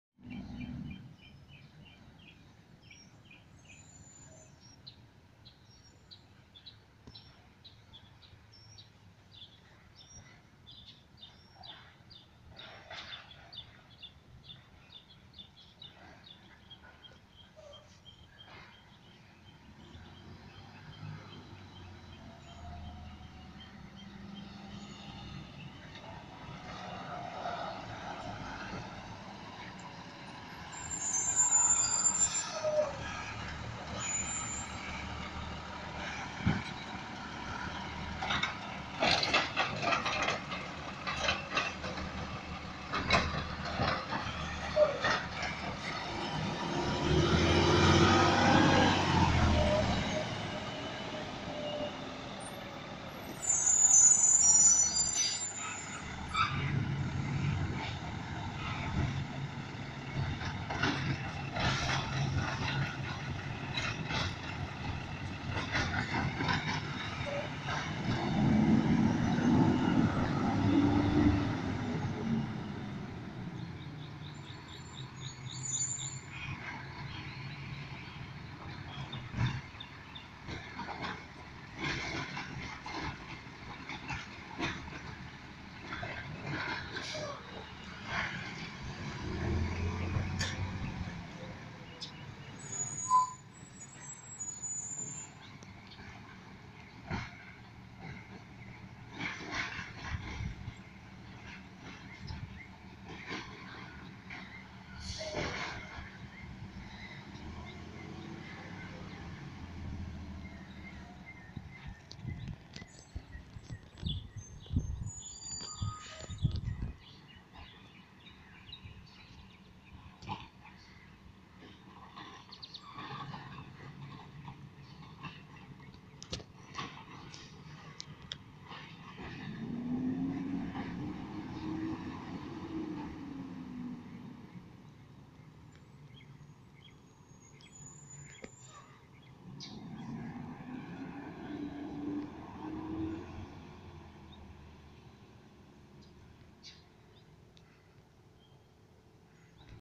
#13201 on garbage part 1
As you know have seen the truck pickup yard waste, recycle and now garbage how lucky I was for Wednesday